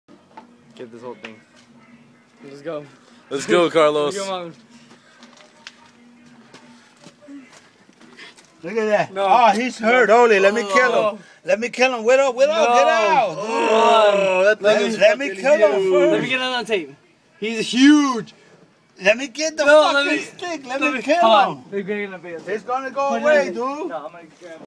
Worlds largest rat ever!
This is a massive rat that's been terrorizing a local farmer. It's been injured.. Will Carlos kill him?